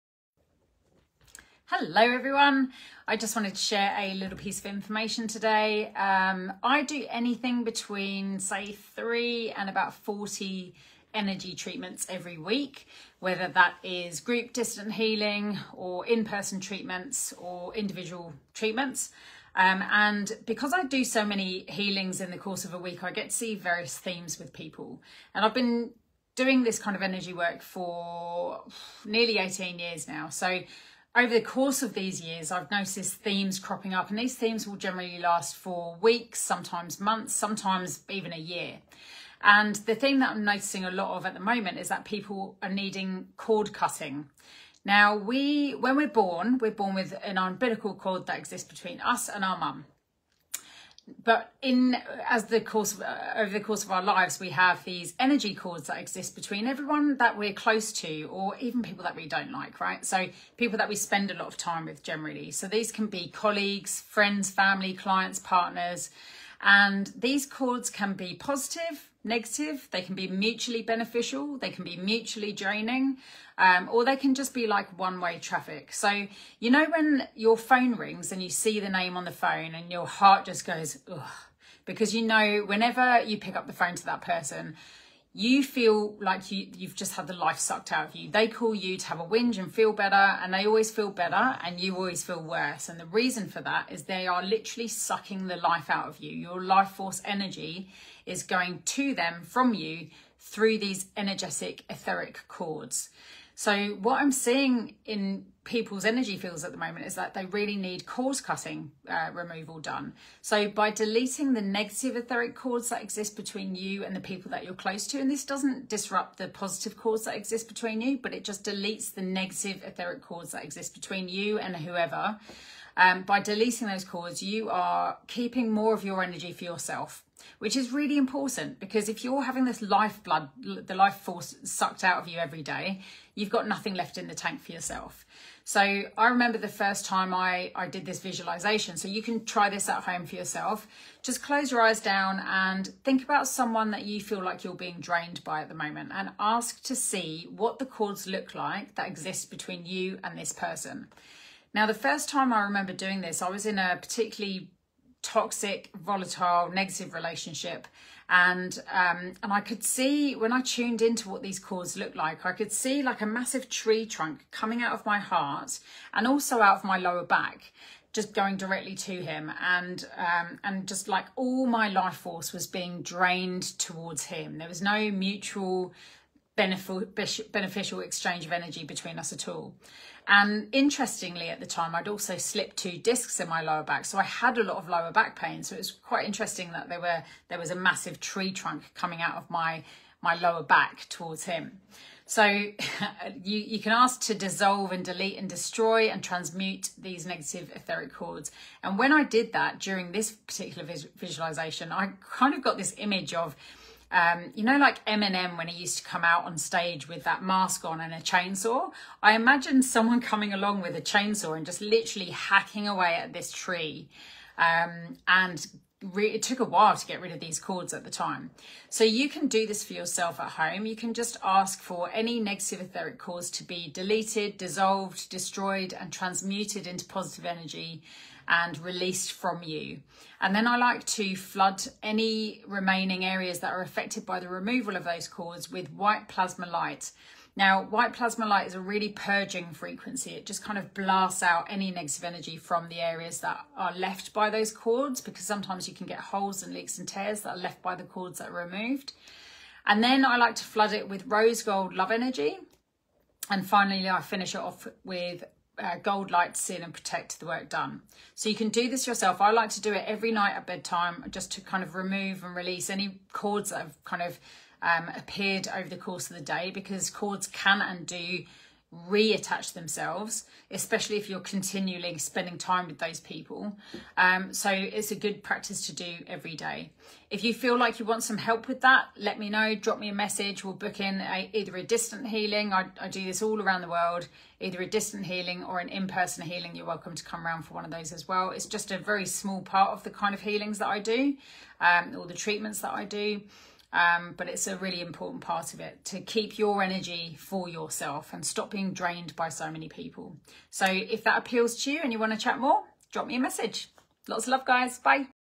The importance of cutting energy cords
Each week I see anywhere between three and forty people for energy work, either in person or via remote healing. 🙌🏻

Because of the amount of people I'm working on each week I notice themes occuring in my clients.

These themes can last weeks or even months.

One of the themes I'm noticing right now is that people are needing cord cutting.

Just like when we're born with an umbilical cord attaching us to our mums, we also have etheric (energy) cords that exist between us and anyone we're close to or spend a lot of time with.

Even if we end a relationship and don't see that person anymore, there are still etheric cords existing between us and if it was a bad break up those cords are generally negative, toxic and draining.

Cords aren't always negative - the cords that exist between us and the important people in our lives can be mutually positive, mutually draining, or like one way traffic.

You know when your phone rings and you see the name, and your heart sinks? 🫣

Yeah.

That's a sign of a draining etheric cord.

That person will call you, complain to you, and literally suck the life out of you through the cords linking you.

I find it's important to delete/dissolve the negative etheric cords that exist between us and anyone in our lives.

It doesn't delete the positive cords, and it frees up your energy to stay within your own field rather than being sucked away by energy vampires.

I find I'm doing this more and more in client sessions.

You can even do this yourself.

Think about someone who's in your life right now who you feel you may be drained by.

Visualise them in front of you and ask to see the etheric cords that exist between the two of you.

I remember the first time I did this many years ago focusing on my partner at the time.

It was an extremely toxic, volatile and chaotic relationship that was sucking the life out of me.

I saw a massive tree trunk coming out of my heart and out of my lower back going into his body with only my energy going towards him and none of his sustaining me.

Interestingly, I had massive pain in my lower back at the time due to a couple of slipped discs.

When you cut the cords, you stop your energy being drained by them for a period of time; however, they can (and do) frequently come back so it's important to cut cords regularly.

I do this most nights when I go to bed. 🛌

I ask for all negative etheric cords to be deleted, destroyed, dissolved and transmuted and ask for all healing to be in line with my highest good. 😇

I then flood my body with white plasma light (to purge any remaining negative frequencies), then rose gold (love energy) and gold to seal and protect.

The more we can release these cords, the more we protect our energy.

If you feel this is something you need right now and don’t want to do it alone, drop me a message and let’s chat about how energy work can help.

Catcha on the flip side, where we no longer let people suck the life out of us.

Coach Carly xx

CO A C H I N G
T H R O U G H
C H A N G E +
T R A U M A ❤️